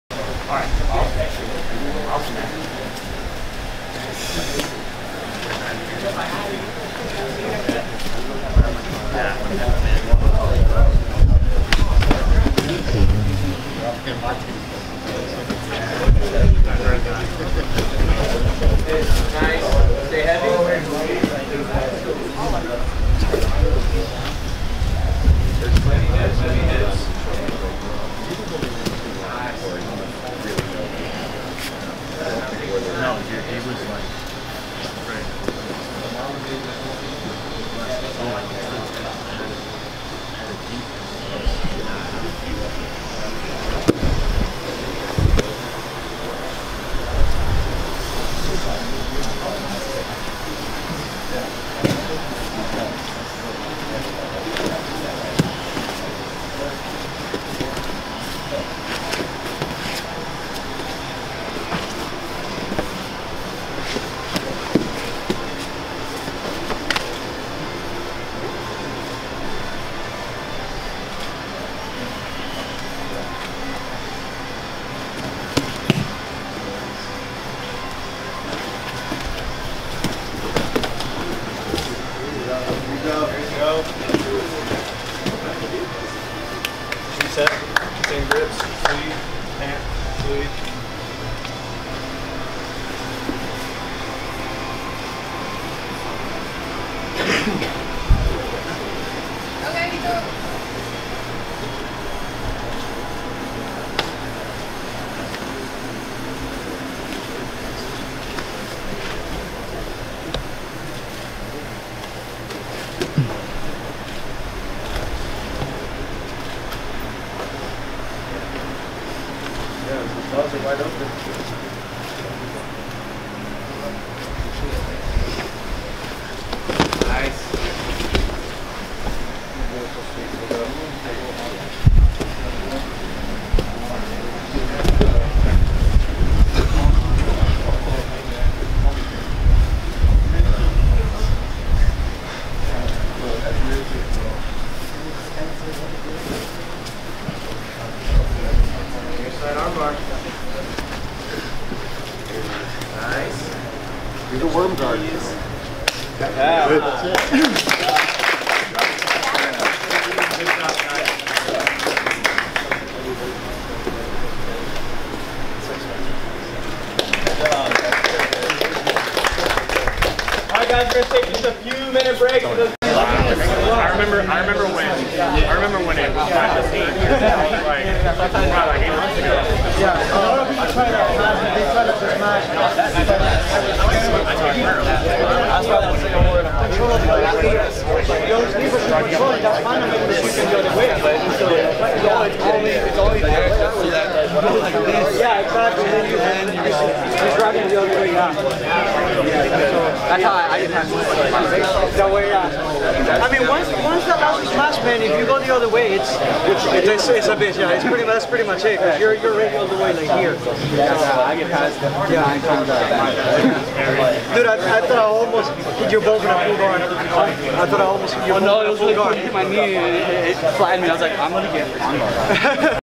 Sankaku Pro Grappling - Michael Lugo vs Antonio Claussen (Lugo via Armbar)
Kagan Pierce submits Blair Forcet to win his submission-only super match.

Support Michael Lugo:
YouTube & Instagram: @mikelugobjj

Sankaku Pro Grappling was a sub-only grappling event featuring the best grapplers around Tampa Bay. Organized by Mike Vizza.